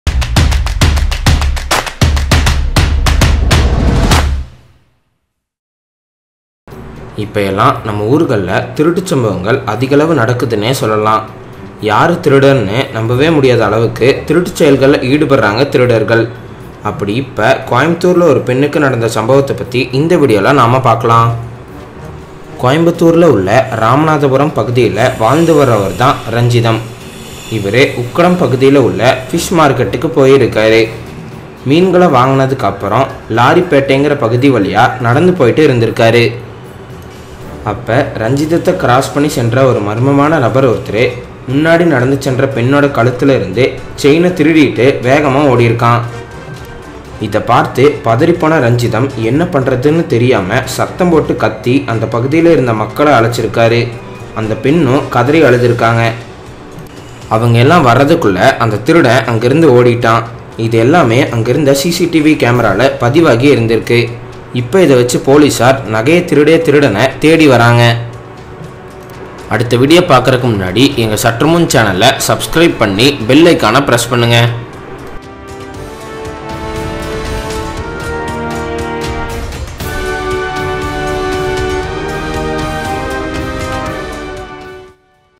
சற்றுமுன் கோவை நடுரோட்டில் பெண்ணுக்கு நடந்த பயங்கரம் | Tamil News | Tamil Seithigal
See what happened to this women in coimbatore main road!

சற்றுமுன் கோவை நடுரோட்டில் பெண்ணுக்கு நடந்த பயங்கரம்

In this video we are going to see what happened to this women in coimbatore main road.